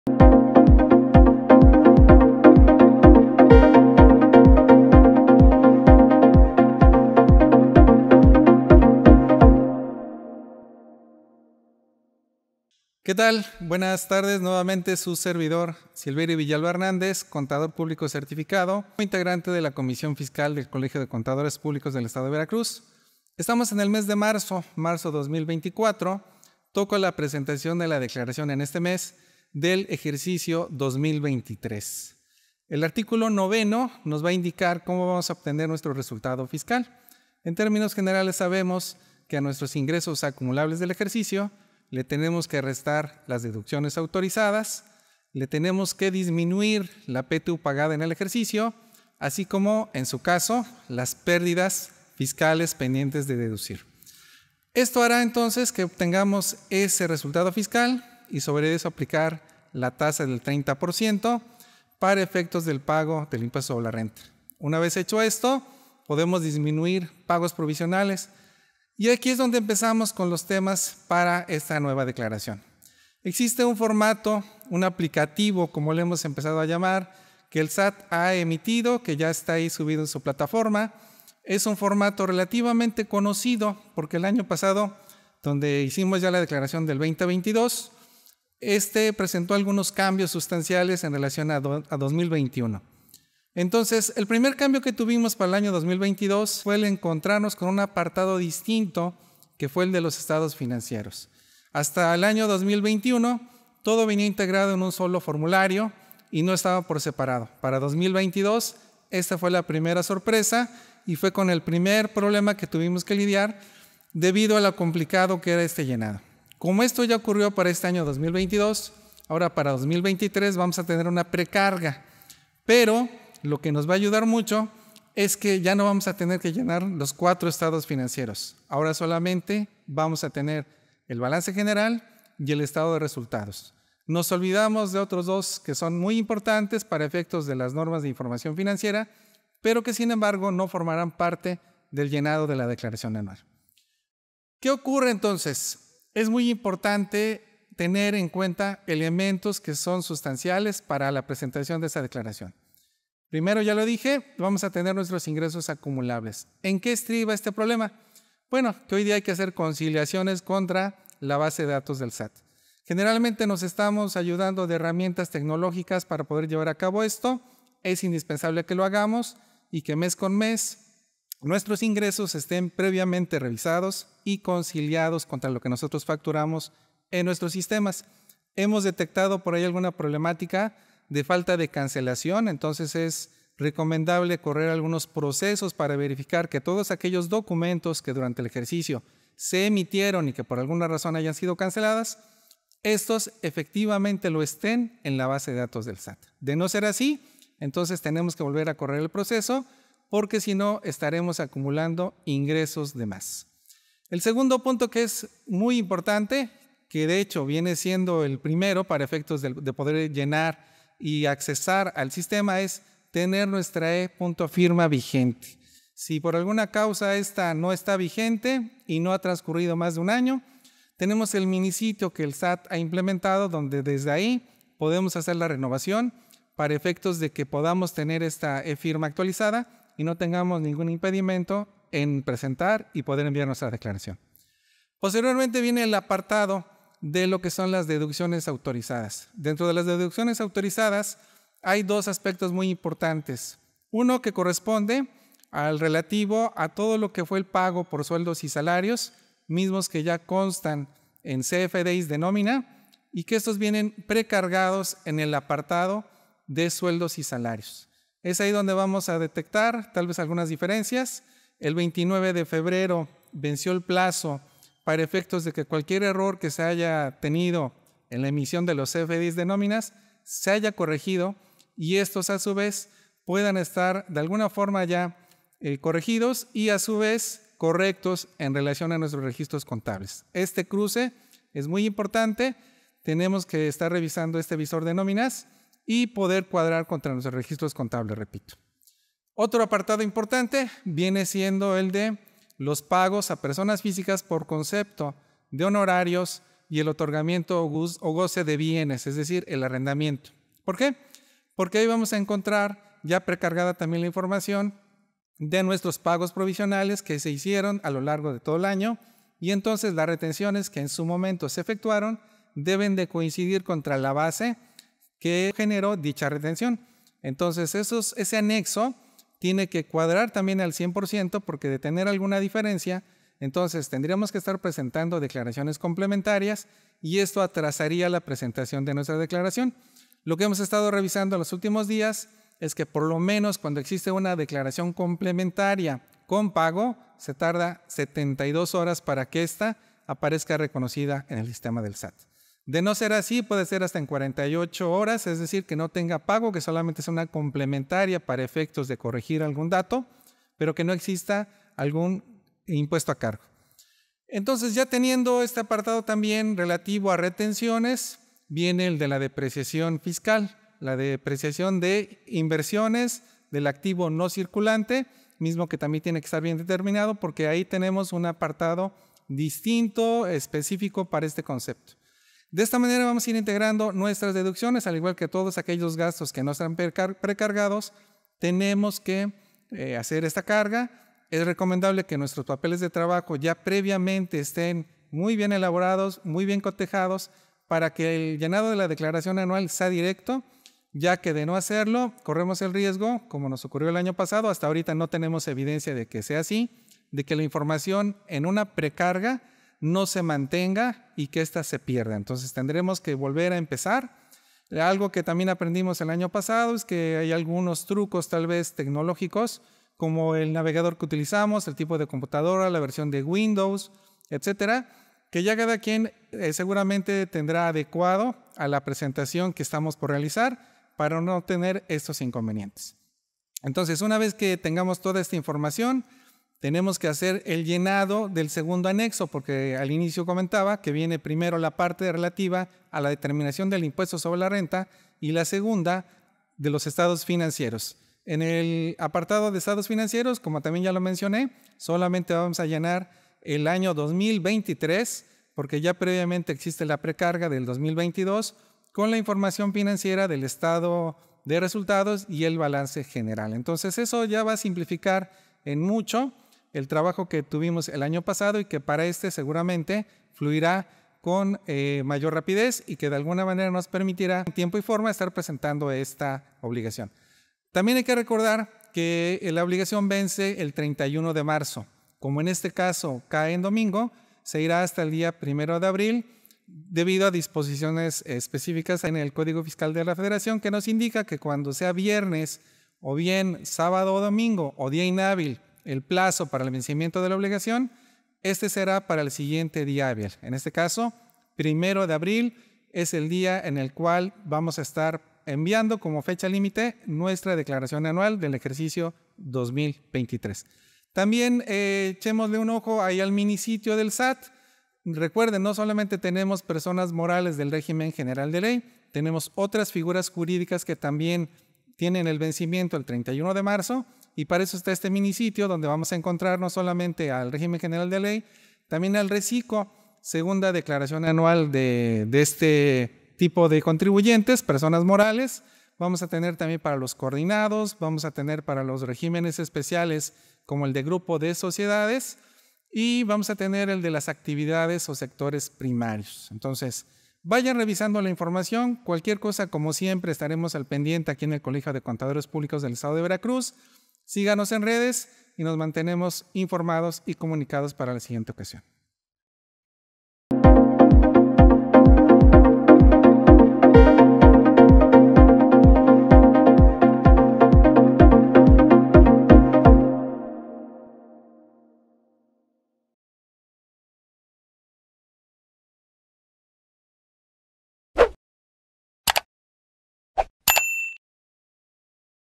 Declaración Anual 2023 para Personas Morales: Guía Completa | CCPVeracruz
En este video esencial, el C.P.C.y M.I. Silverio Villalva Hernández,  integrante de la comisión fiscal del Colegio de Contadores Públicos del Estado de Veracruz, te guía a través del proceso de presentación de la Declaración Anual 2023 para personas morales. Con la llegada de marzo 2024, es crucial entender cómo realizar correctamente esta declaración, aprovechando las novedades y evitando los errores comunes.

Descubre cómo obtener tu resultado fiscal aplicando deducciones autorizadas, manejar la PTU pagada, y ajustar por pérdidas fiscales pendientes. Aprende sobre la tasa del 30% para el pago del impuesto sobre la renta, la importancia de mantener tu e.firma vigente, y cómo las tecnologías actuales facilitan conciliaciones con la base de datos del SAT.

Este año, la presentación se simplifica al solo requerir el balance general y el estado de resultados, dejando atrás otros estados financieros. Además, Sil te ofrece consejos prácticos sobre cómo prepararte mejor para este proceso, incluyendo la revisión de sueldos, salarios, y deducciones autorizadas.

El video también cubre aspectos técnicos importantes, como la precarga de información del año anterior y cómo asegurar que tus declaraciones complementarias se reconozcan en tiempo. No te pierdas los insights sobre cómo los ajustes tecnológicos y la elección de tu navegador pueden influir en el éxito de tu presentación.

Recuerda, el plazo vence el 31 de marzo, pero este año se extiende hasta el 1 de abril. Mantente informado y preparado con nuestra guía experta y evita sorpresas de última hora.

Tu éxito en la declaración anual es nuestra prioridad.